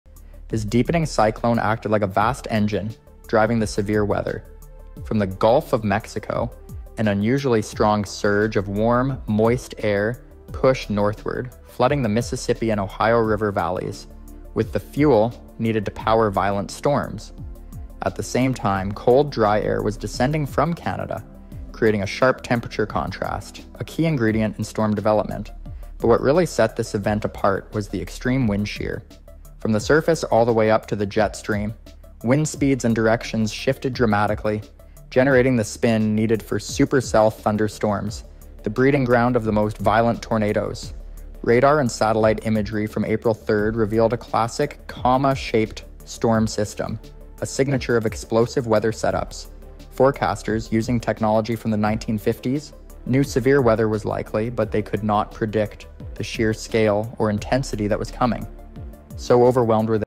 3 HORRIFYING Tornado Outbreaks That Destroyed America!
3 HORRIFYING Tornado Outbreaks That Destroyed America! Part 4

This video dives into three of the most catastrophic tornado outbreaks ever recorded in modern history. From massive EF4 and EF5 tornadoes to widespread destruction across multiple states, these deadly events left lasting marks on the communities they tore through. You'll witness the terrifying power of nature through true stories, expert insight, and real tornado footage. This is not just a weather documentary — it's a chilling reminder of the force behind extreme weather events. Packed with details on tornado paths, storm development, and emergency response, the video highlights just how unpredictable and devastating severe weather can be.

If you're interested in tornado history, storm chasing, or natural disaster preparedness, this is the ultimate video for you. Learn how these outbreaks changed meteorology and emergency response systems across the country. Whether you're here for storm footage, tornado survival stories, or educational content, this compilation has it all.